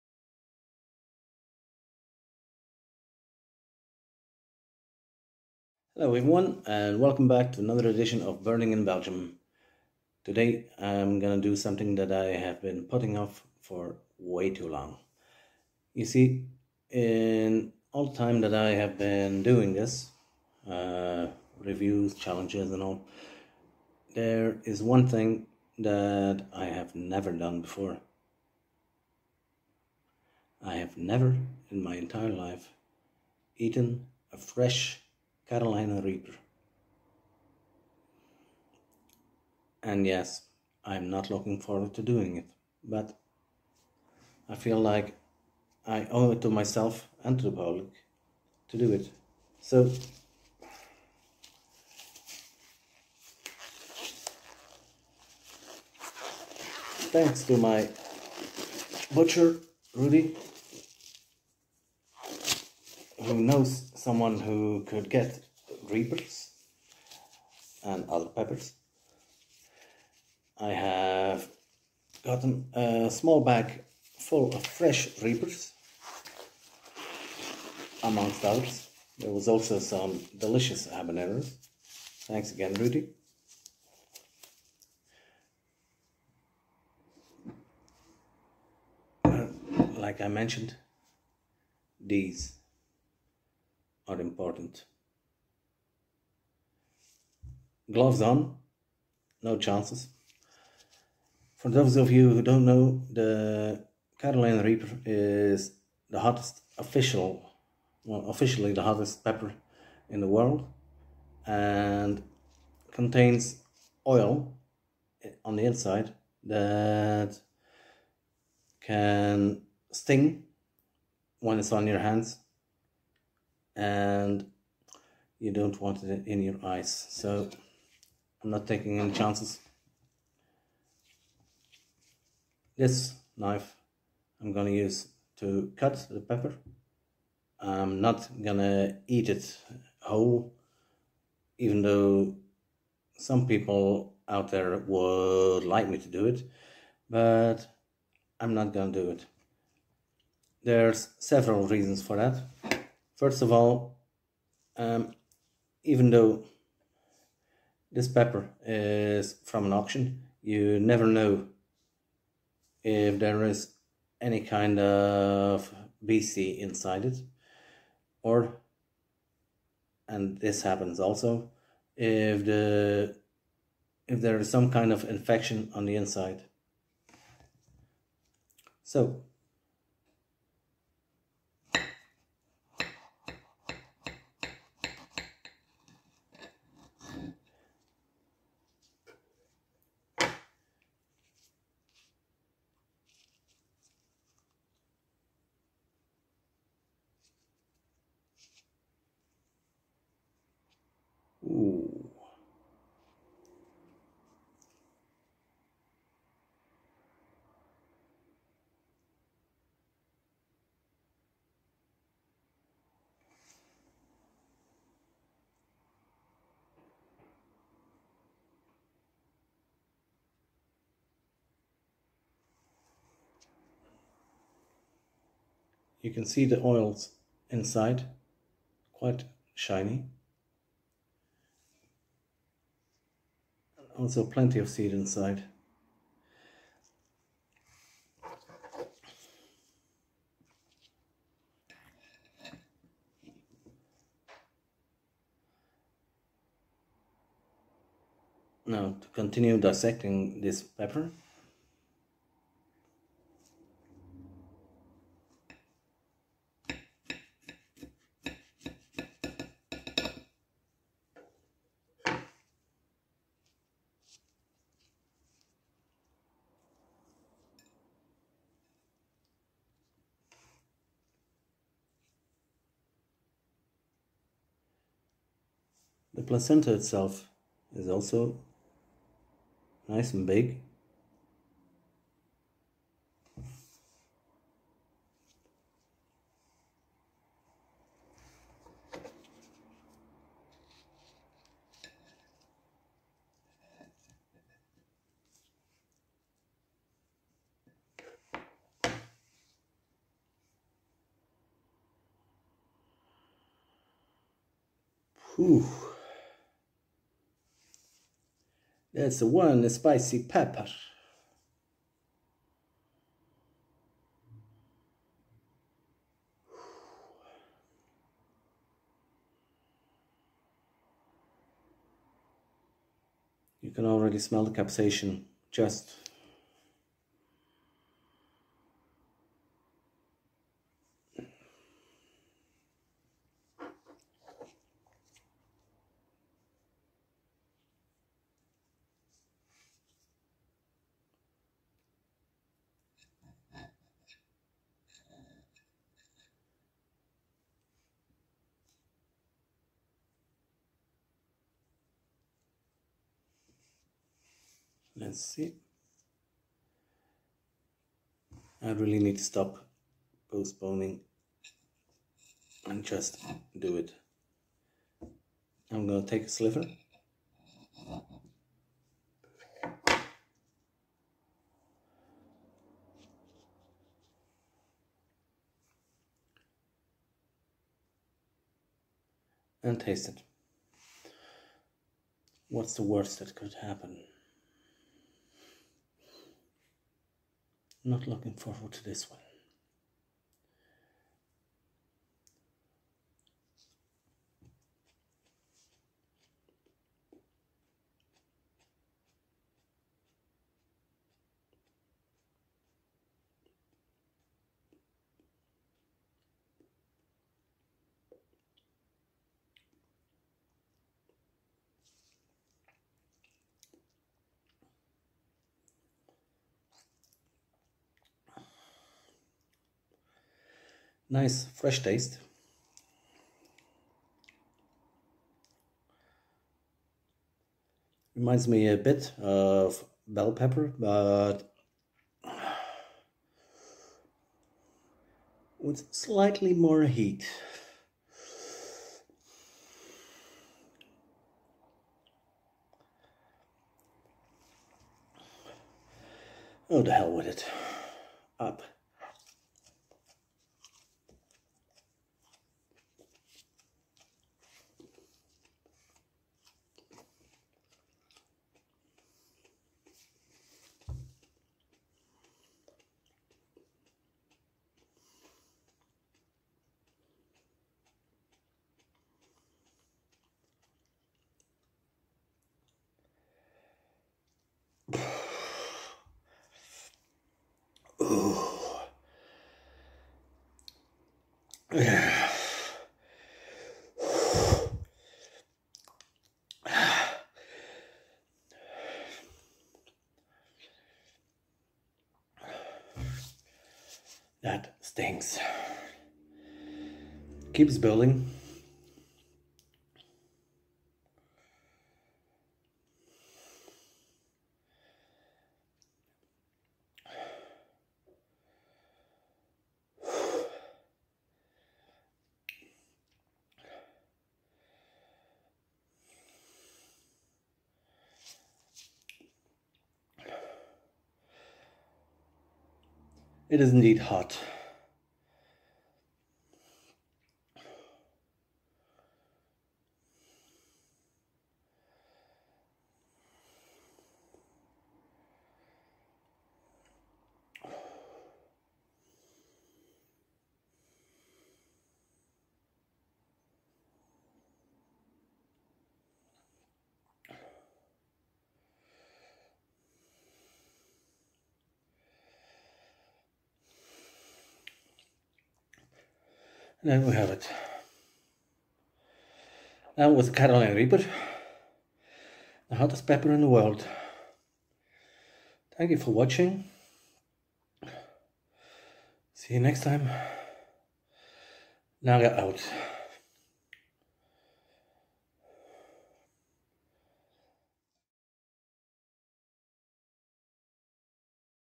Burning in Belgium review: Don't fear the reaper!
Finally, I've been able to get some fresh reapers.
Hope you enjoy watching me suffer.